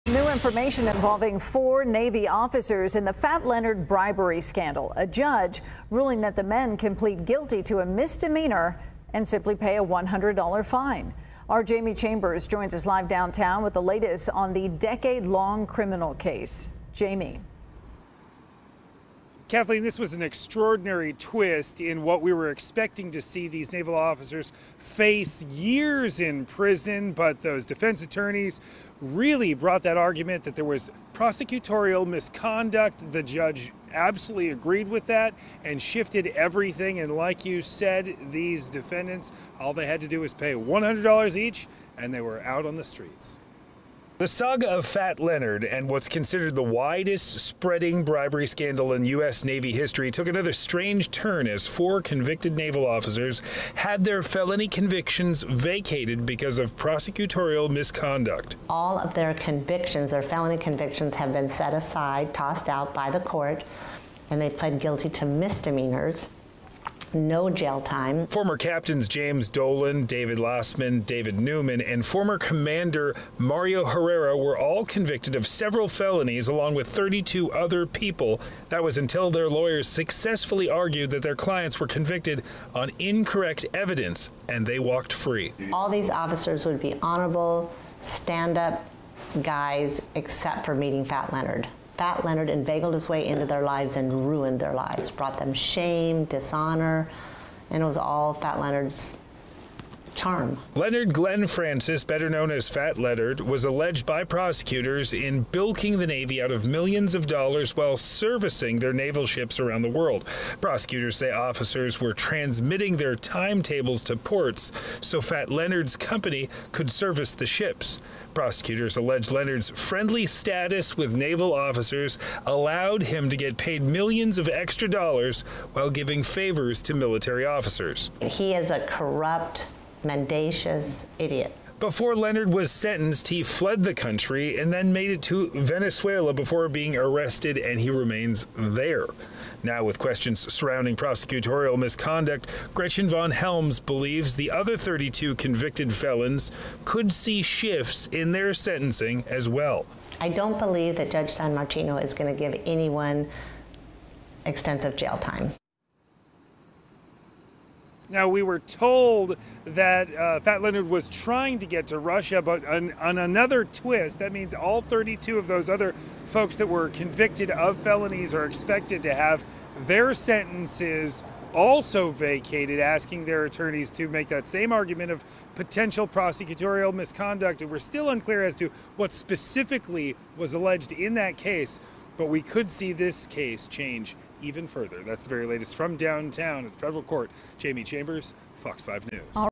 Felony Convictions Vacated For Ex-Navy Officers In 'Fat Leonard' Scandal
The felony convictions of four former Navy officers were vacated Wednesday.